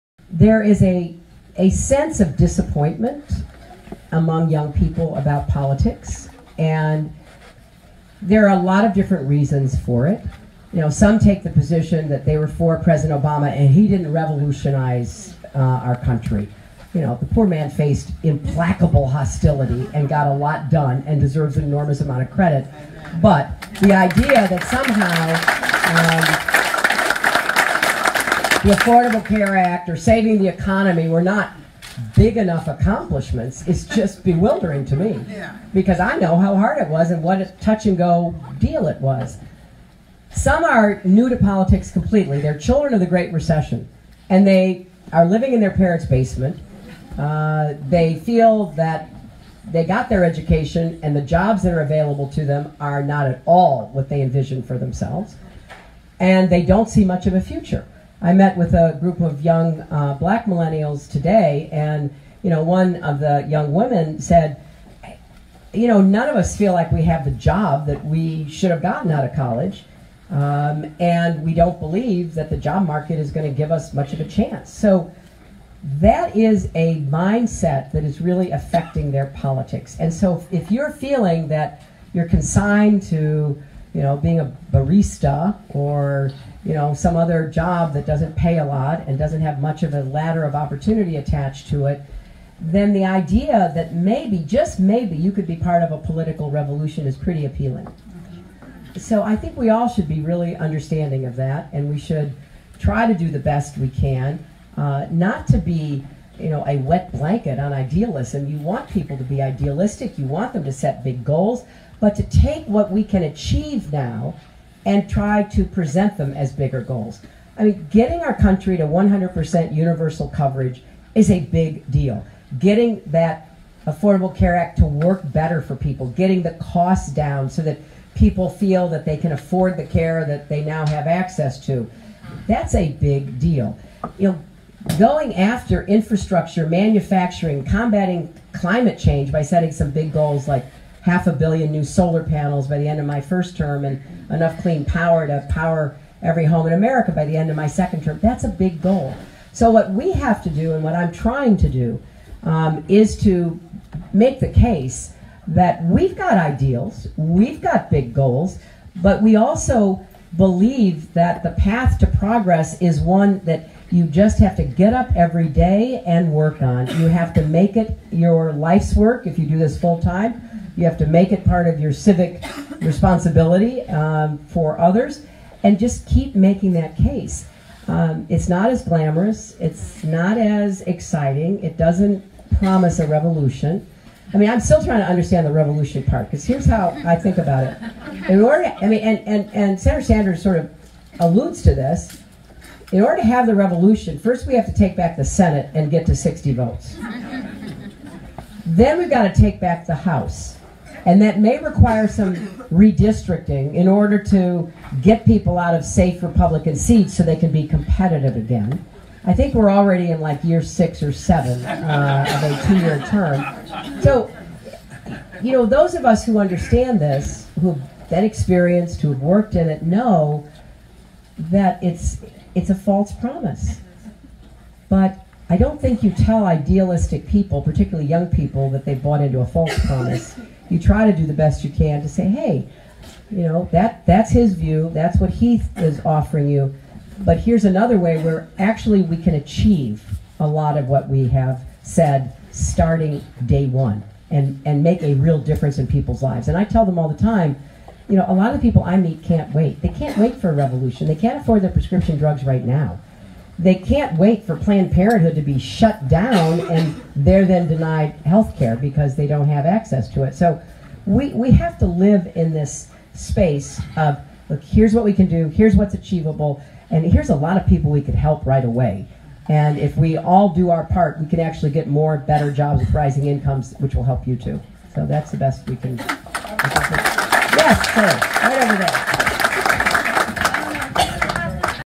Hillary on Bernie Supporters at Private Event in Virginia
“Some are new to politics completely. They’re children of the Great Recession and they are living in their parents’ basement. They feel they got their education, and the jobs that are available to them are not at all what they envisioned for themselves, and they don’t see much of a future,” Clinton said. “I met with a group of young black millennials today, and, you know, one of the young women said, ‘You know, none of us feel like we have the job that we should have gotten out of college, and we don’t believe the job market is going to give us much of a chance.’”
“So that is a mindset that is really affecting their politics, and so if you’re feeling like you’re consigned to, you know, being a barista or, you know, some other job that doesn’t pay a lot and doesn’t have some other ladder of opportunity attached to it, then the idea that maybe — just maybe — you could be part of a political revolution is pretty appealing. So I think we should all be really understanding of that and should try to do the best we can not to be, you know, a wet blanket on idealism — you want people to be idealistic; you want them to set big goals — but to take what we can achieve now and try to present them as bigger goals.”